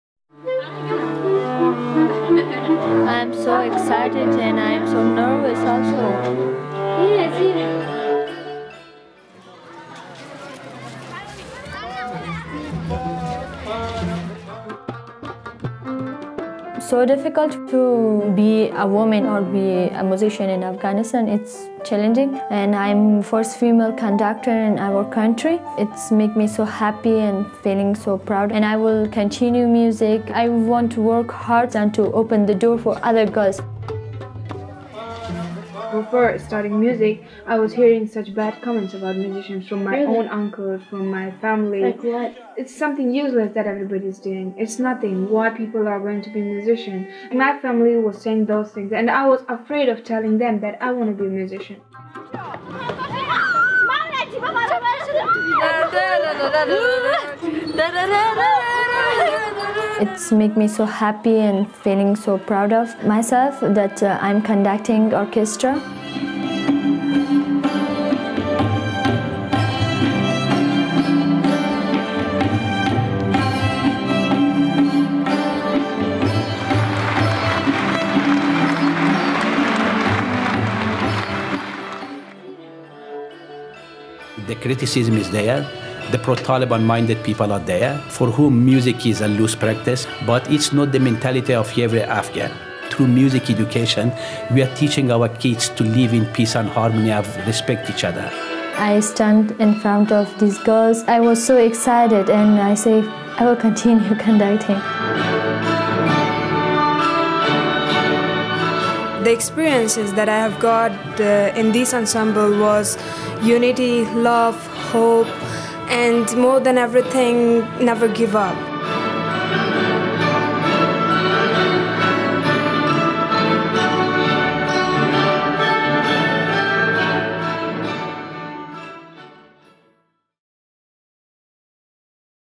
Afghanistan’s All-Female Orchestra Goes Global
In spite of fierce resistance at home, a brave group of Afghan girls has come together to form Zohra -- an all-female orchestra named after a Persian goddess.  Now this musical group is taking their message of hope and empowerment overseas.